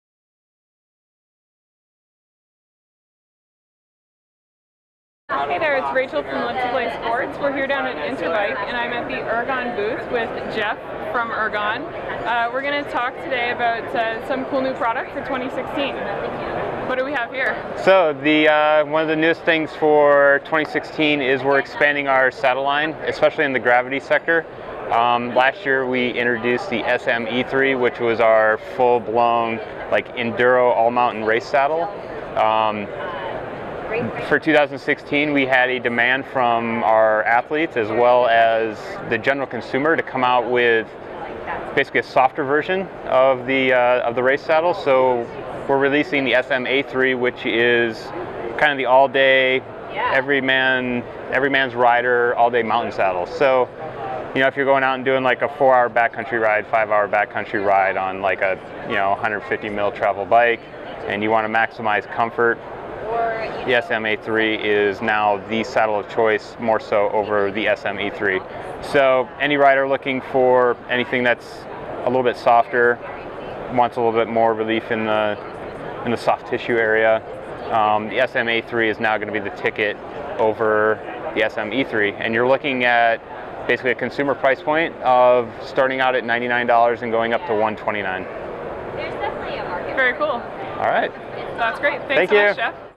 Interbike 2015 - Ergon - SMA3 saddle
Live to Play Sports visits the Ergon booth at Interbike 2015 to learn about what is new for 2016.